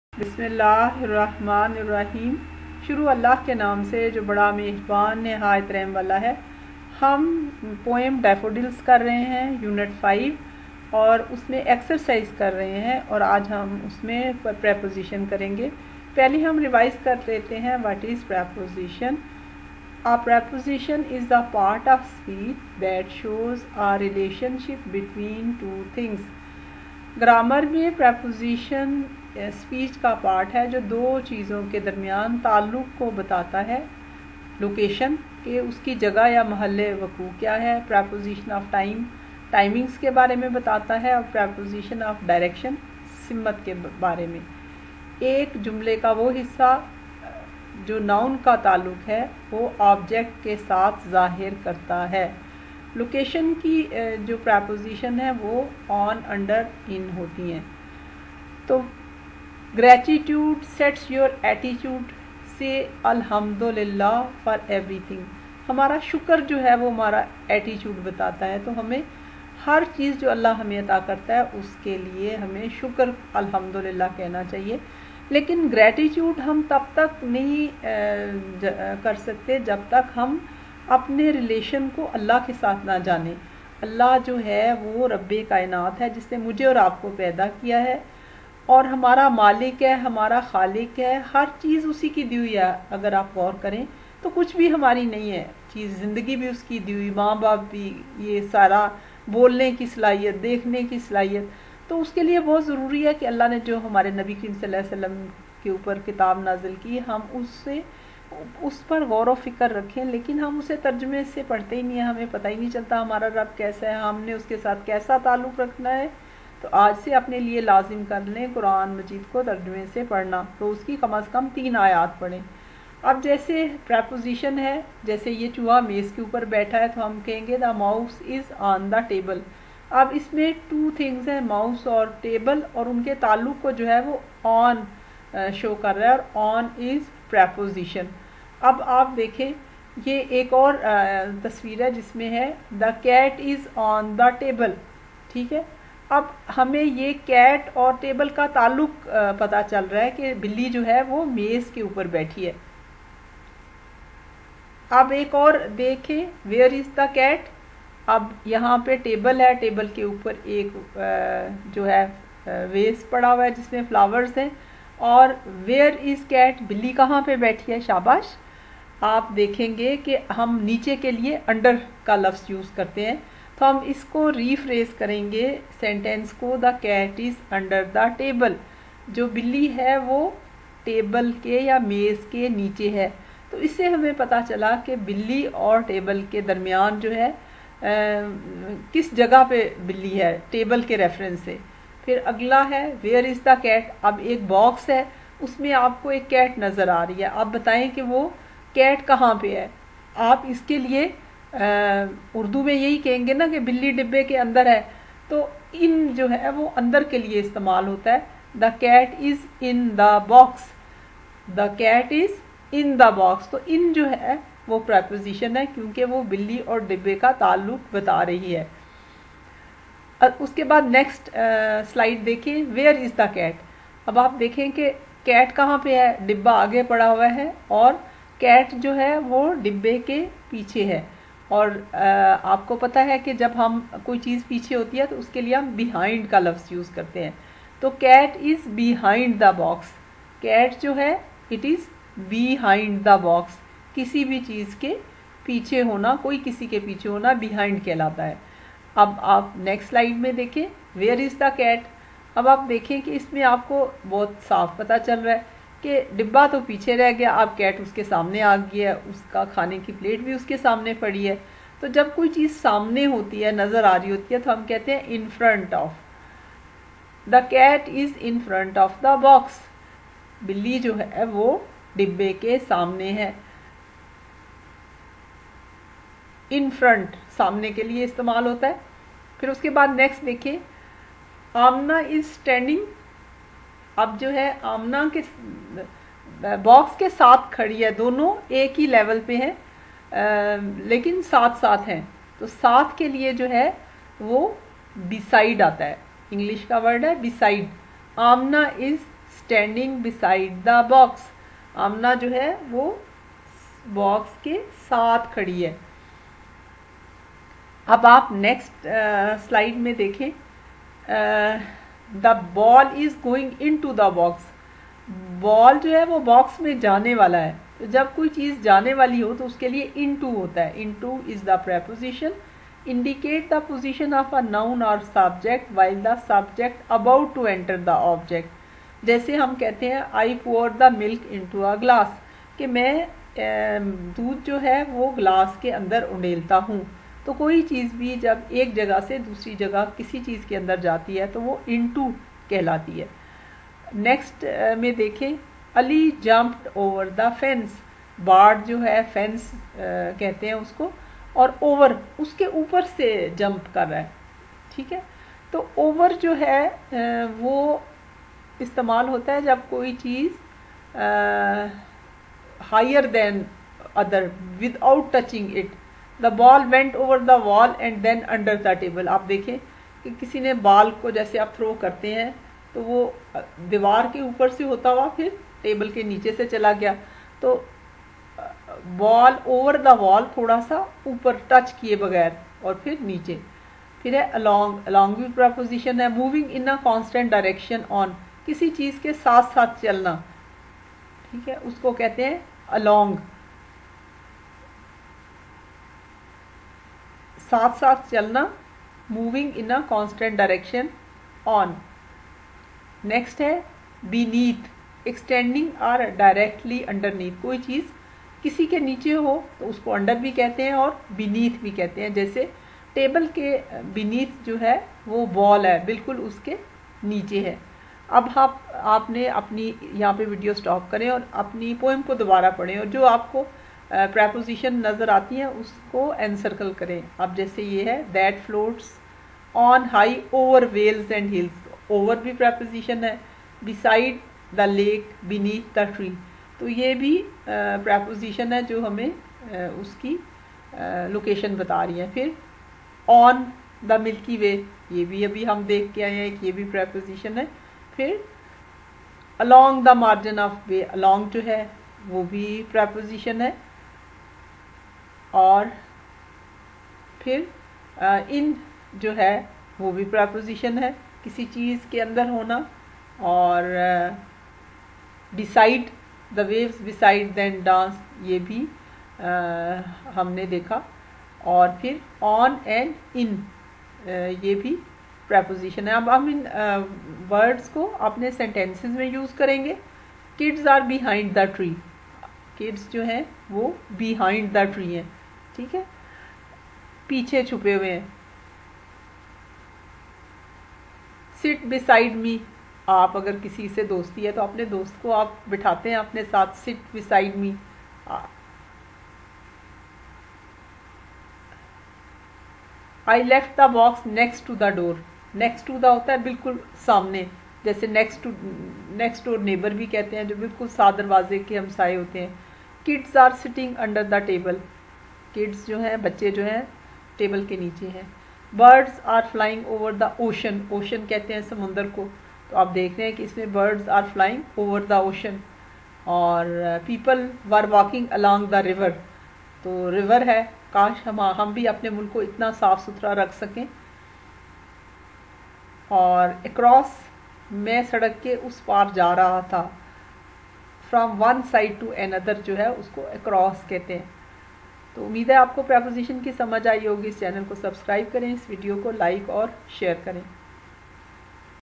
English 9 in Urdu/Poem daffodils/English grammar/Preposition/unit 5.10
9th English unit 5 Lesson 10, Exercise Question identify preposition in Urdu